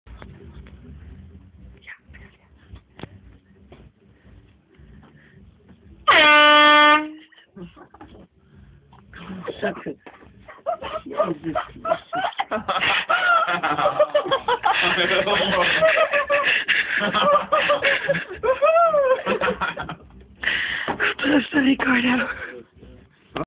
wakker worden
riccardo trekt het niet meer en gaat om 1 uur nachts al naar z`n bed, terwijl heel maastricht nog carnaval viert...